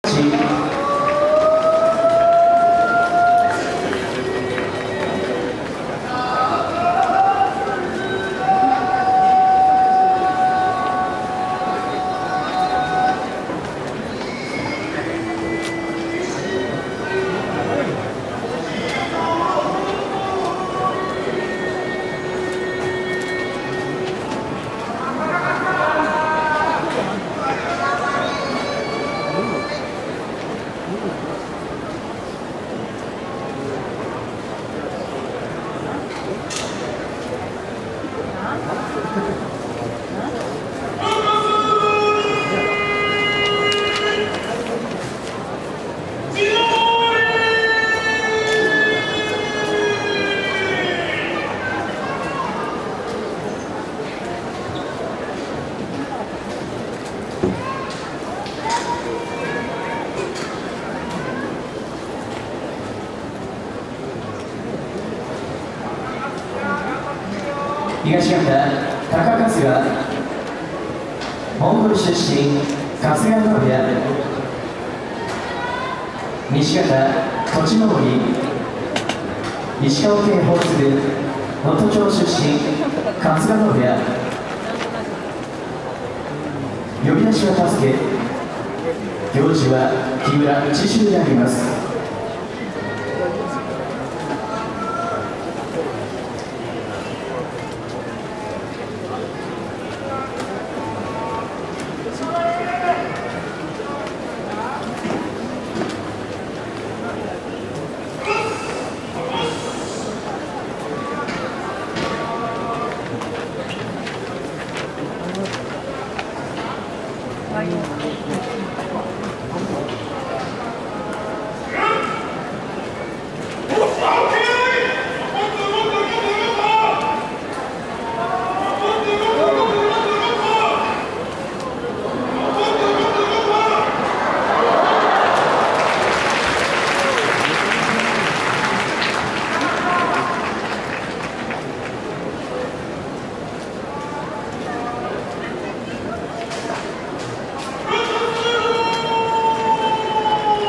大相撲.2016.7.12㉔/高春日(takakasuga)-栃登(tochinobori)/nagoya Basho/day 3/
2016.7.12の取組み動画です。
大相撲本場所の幕下&十両の取組を中心に自分で撮影した動画をアップしています。